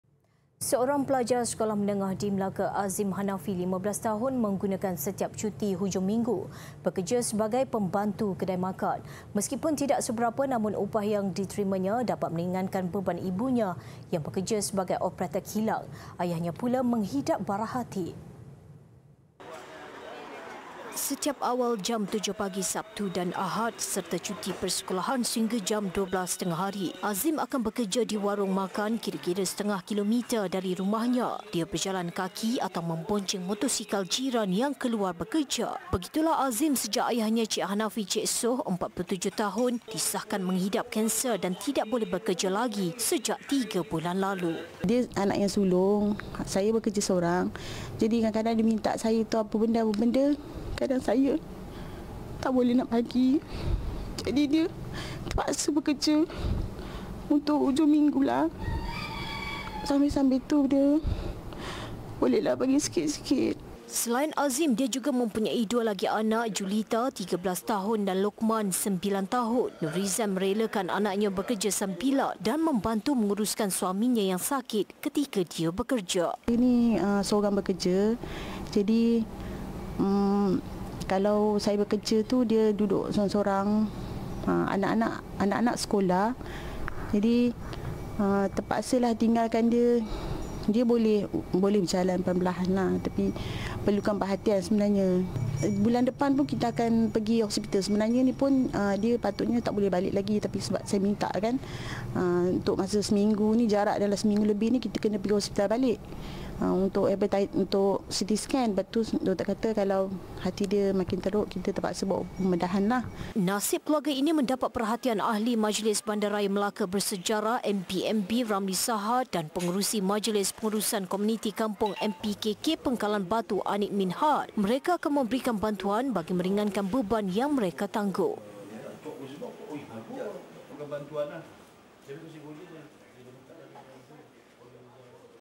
PRIHATIN | Remaja 15 Tahun Bantu Sara Keluarga
Seorang pelajar sekolah menengah di Melaka,  Azim Hanafi, 15 menggunakan setiap cuti hujung minggu dengan bekerja sebagai pembantu kedai makan.

Meskipun upah tidak seberapa, namun pendapatan yang diterimanya dapat meringankan beban ibunya yang bekerja sebagai operator kilang.

Ayahnya pula menghidap barah hati. Semoga urusan keluarga ini dipermudahkan.